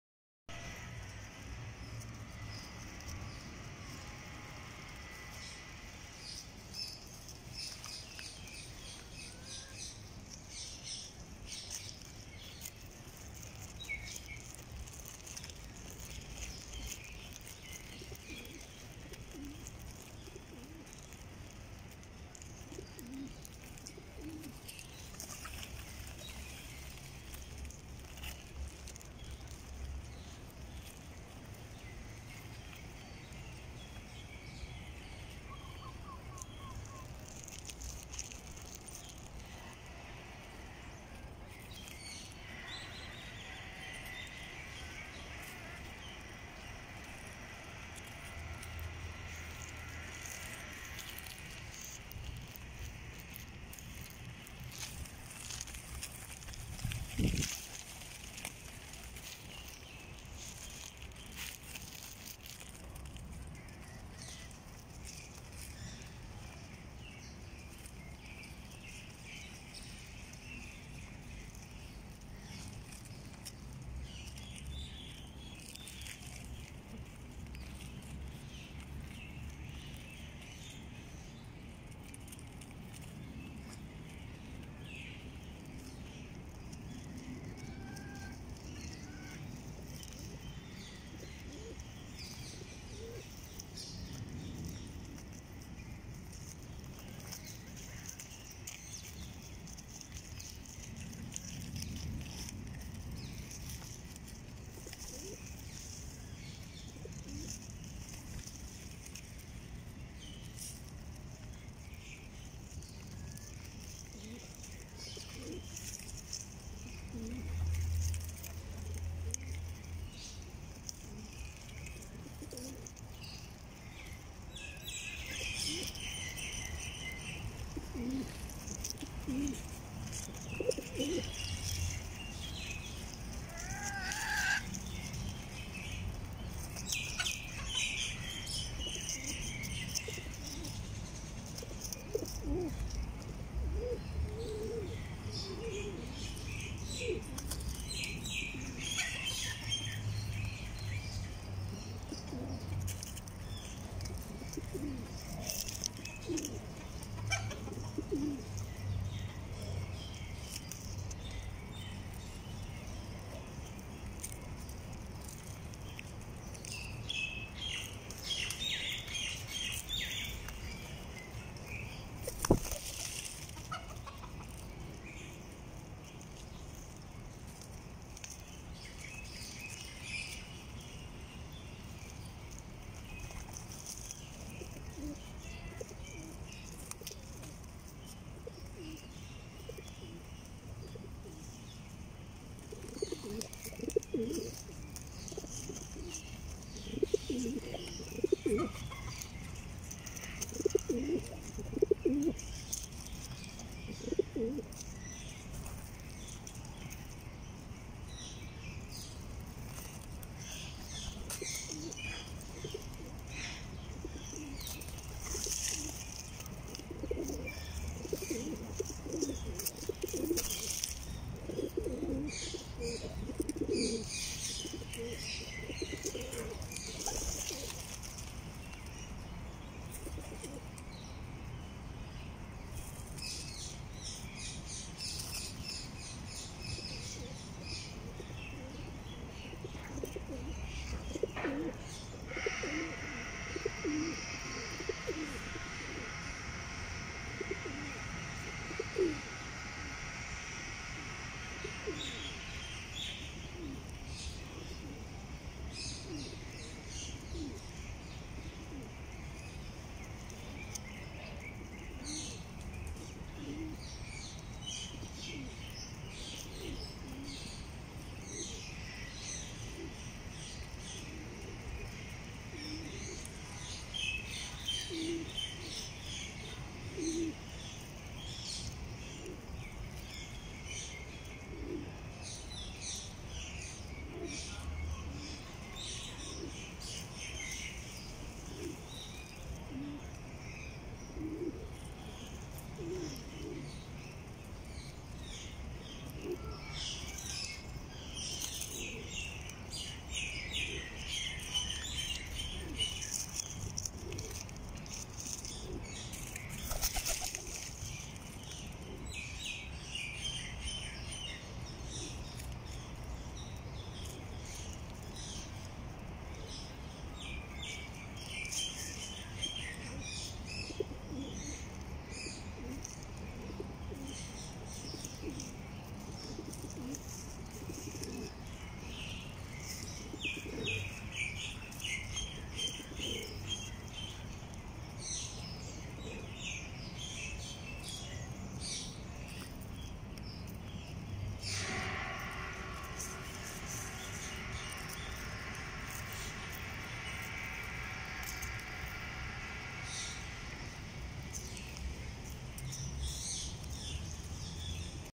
欢快的一天 A Happy day！
鹦鹉空中欢快鸣，鸽鸽争食忙不停，公鸡帅哥最绅士，谨慎视察卫后宫